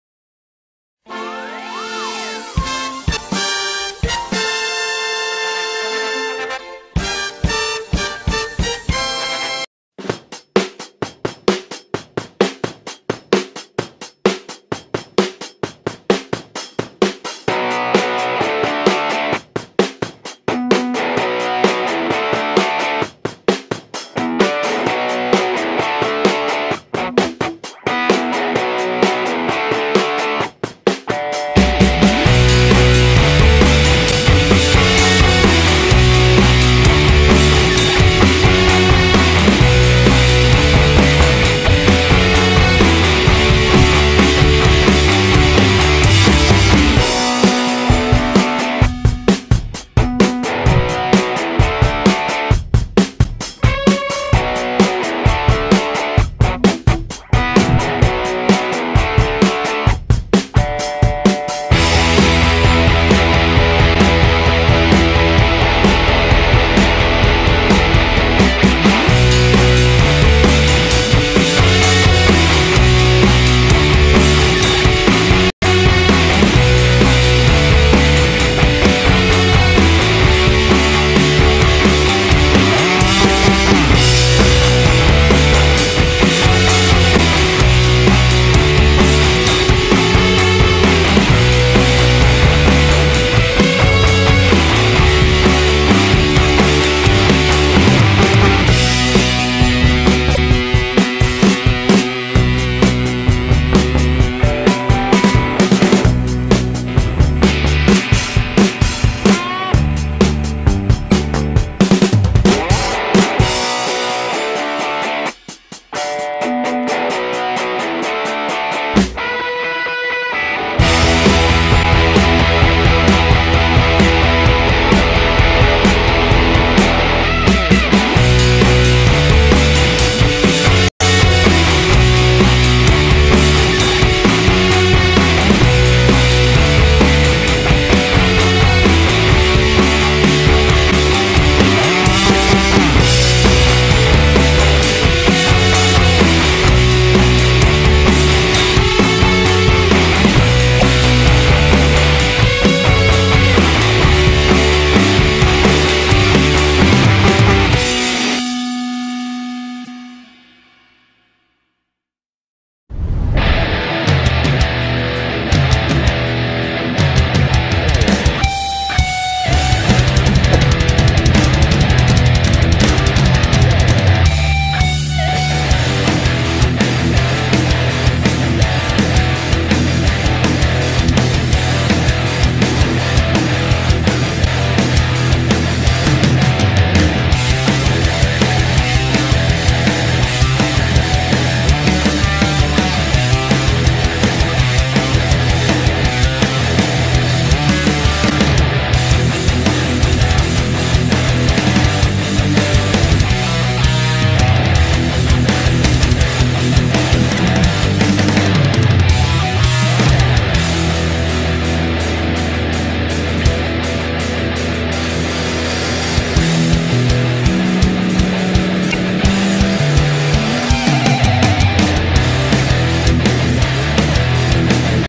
Fortaleza-CE Imagens Aéreas da região central da Cidade (meio)
Sobrevoando o Meio de Fortaleza-CE longe da Praia.